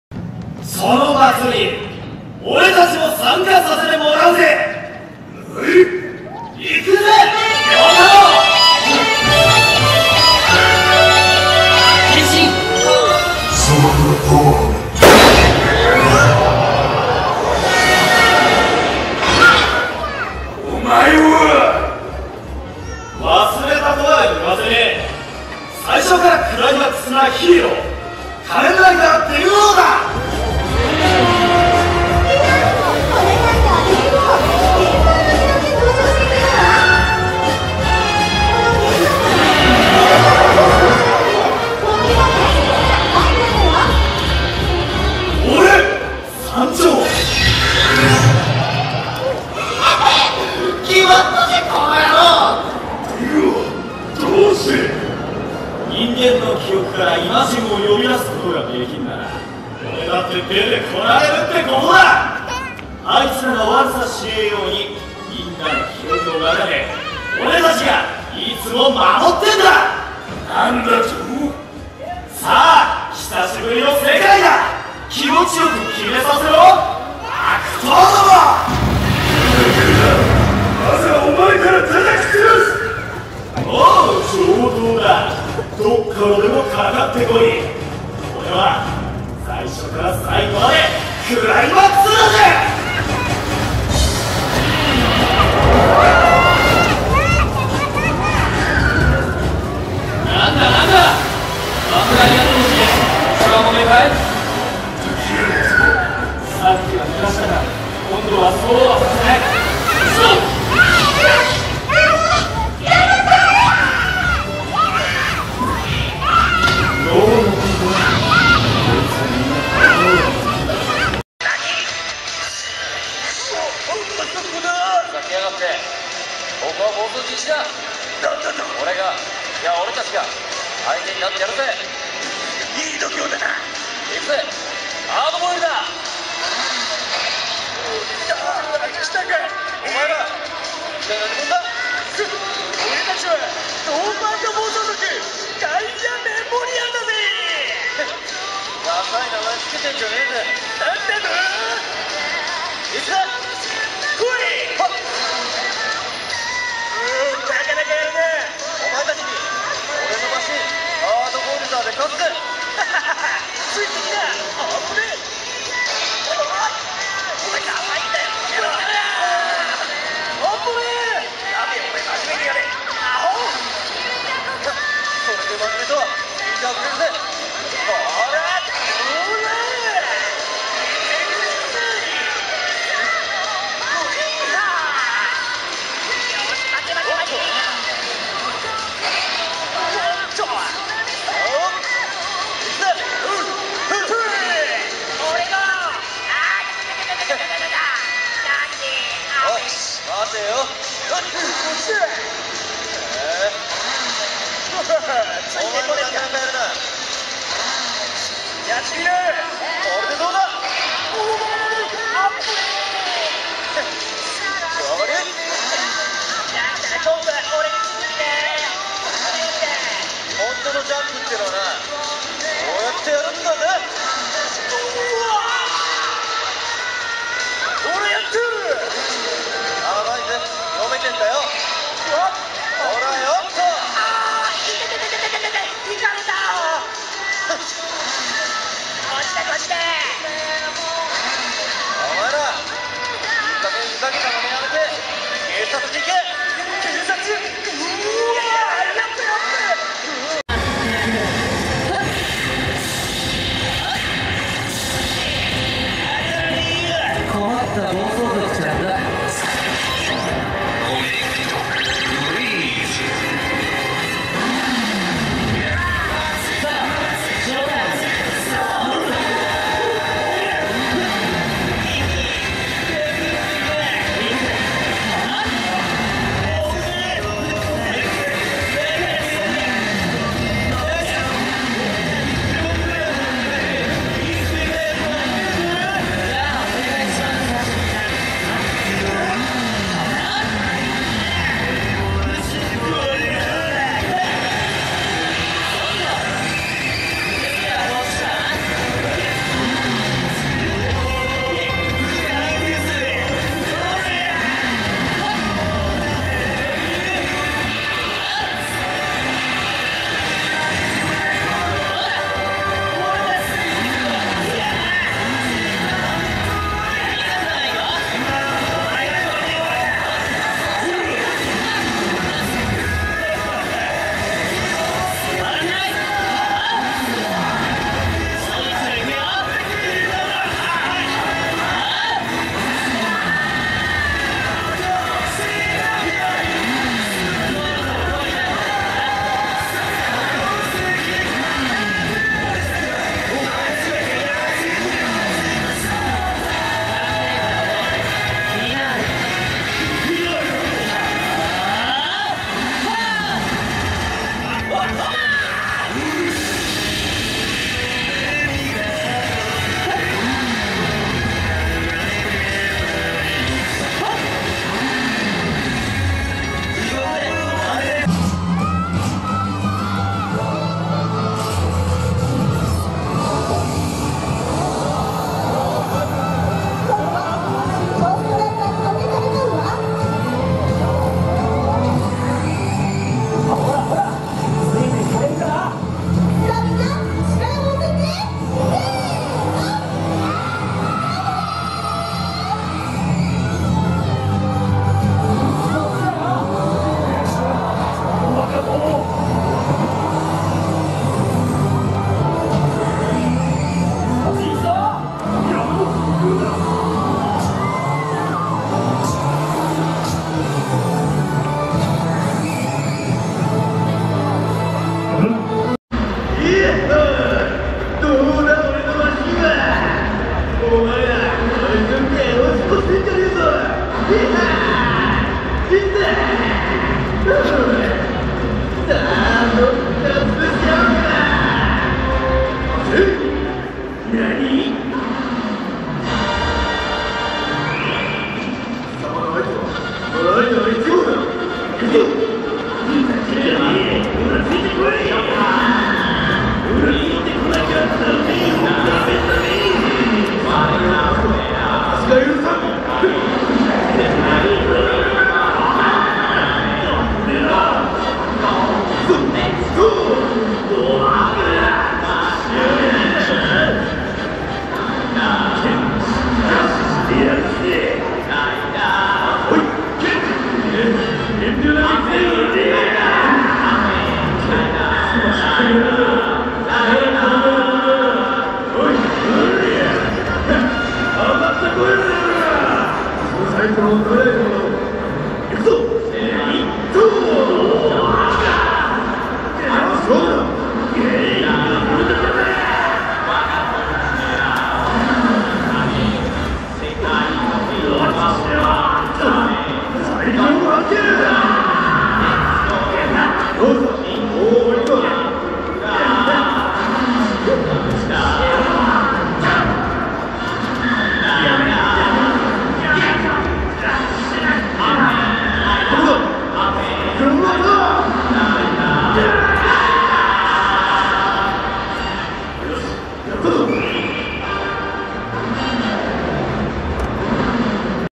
Kamen rider bike stunt liveshow compilation (Ichigo, Den-O, W, Wizard, Ghost)
仮面ライダーバイクスタントライブ